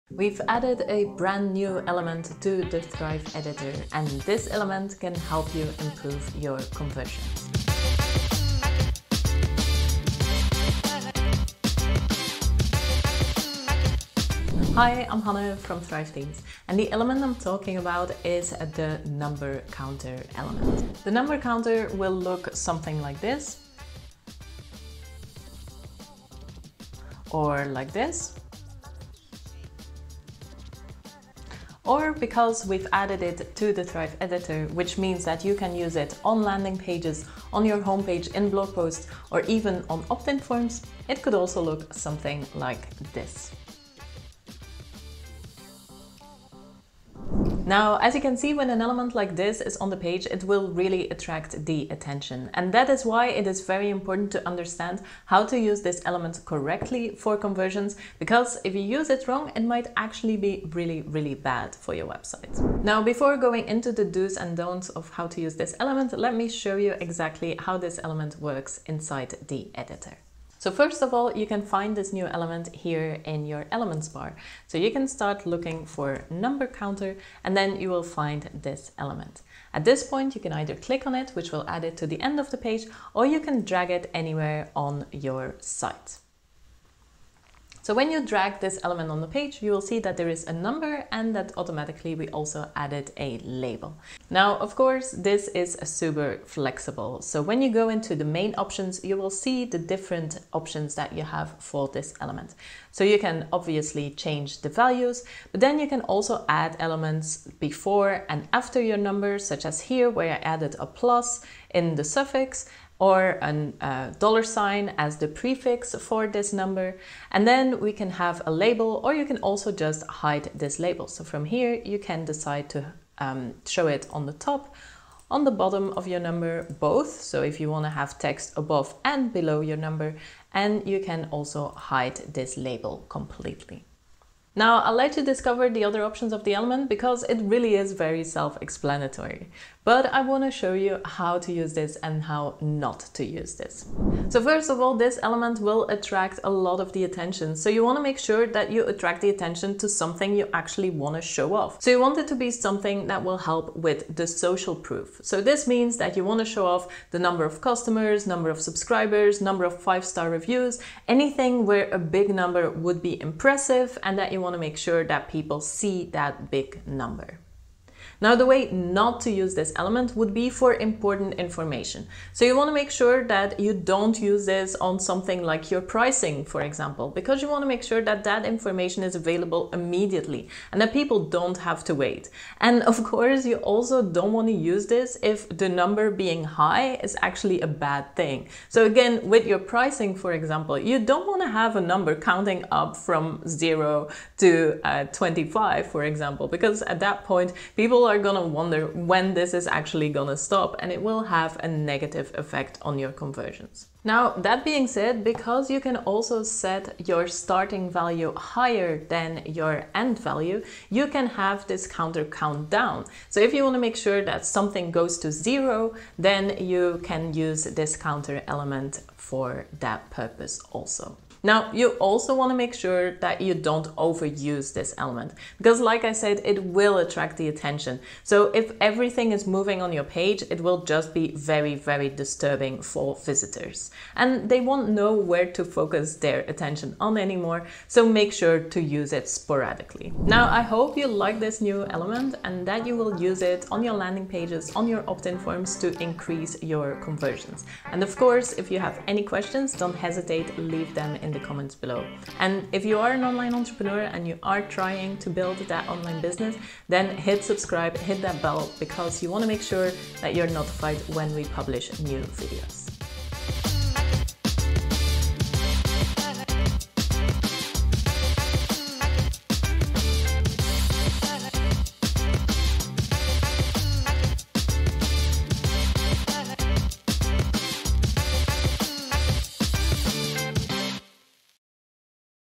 How to Use Thrive Suite's Number Counter Element to Boost Engagement
When you've got impressive stats to show off, number counter animations will help bring your visitor's attention to them.

Watch this video to learn how you can boost conversions using the animated Number Counter element now available inside Thrive Suite's visual editor.

Time Stamps:

00:00 - Intro
00:14 - Animated Number Counter Element Examples
00:47 - How To Use Thrive Suite's Number Counter Element
02:14 - Number Counter Do's
02:47 - Number Counter Don'ts
03:23 - Number Counters Can Also Count Down
03:41 - Don't Overuse Number Counters
03:59 - Make It Count!

Have more questions about how to deploy animated Number Counter elements on your landing pages and inside content?

Just leave them in the comments below so we can get you answers!